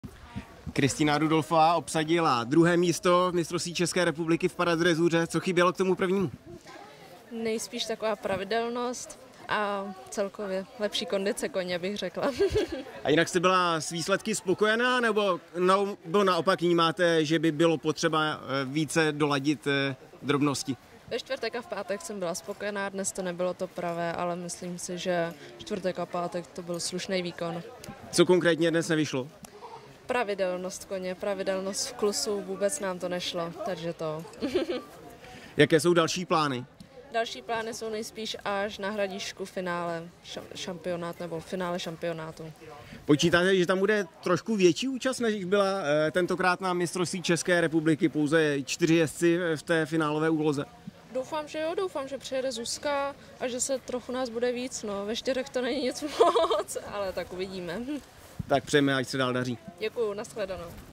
Kristýna Rudolfová – Nedělní soutěže při mistrovství ČR 2019 v drezuře Královice u Slaného
Při nedělním vrcholu mistrovství ČR 2019 v drezuře a paradrezuře bylo v Královicích rozdáno pět sad medailí pro jezdce do 25 let, seniory v malé rundě, starší juniory, handicapované jezdce a seniory ve střední rundě. Koukněte na záznam a rozhovory!